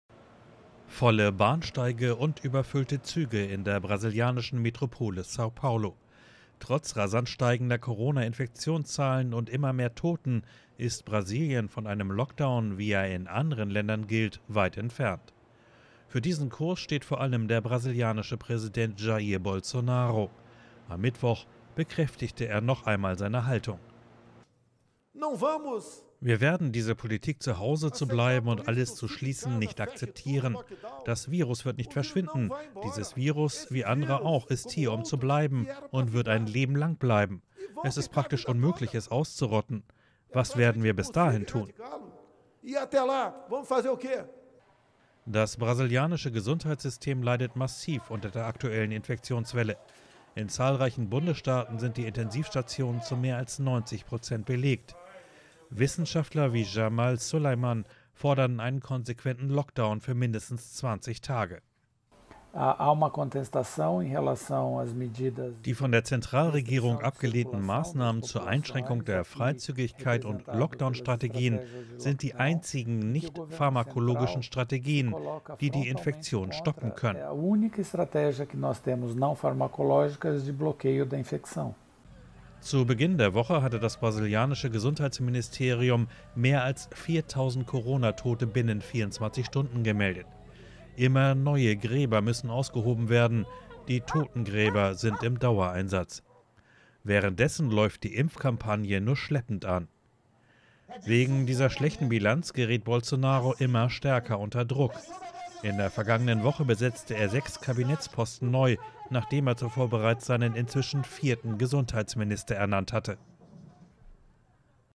Wissenschaftler in Brasilien fordern Lockdown
Trotz rasant steigender Corona-Infektionszahlen und immer mehr Toten lehnt der brasilianische Präsident Jair Bolsonaro einen Lockdown weiter ab. In zahlreichen Bundesstaaten sind die Intensivstationen zu mehr als 90 Prozent belegt.